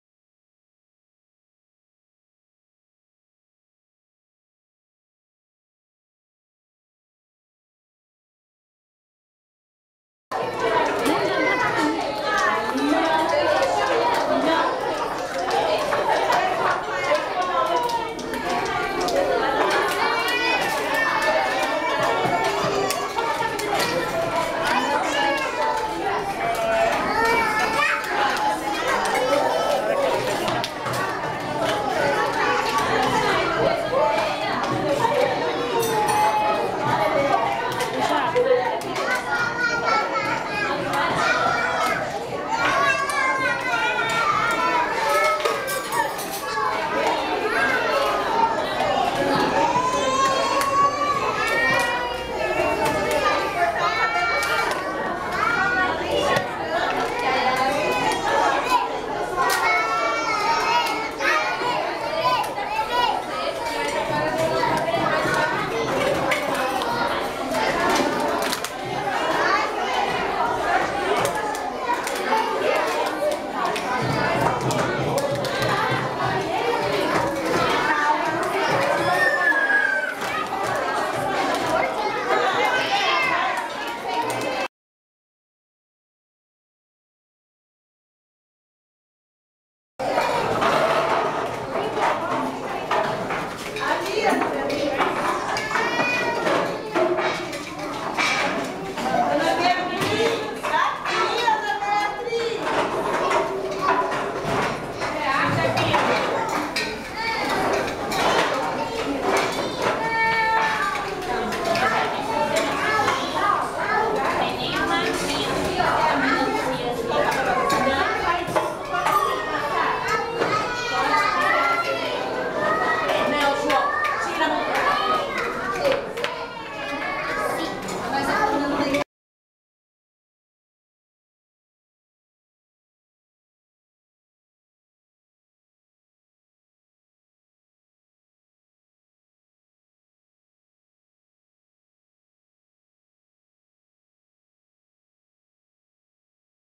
VIDEO LUCAS NA CRECHE DIA 280717
Gravação no nosso filho Lucas na creche no dia 280717.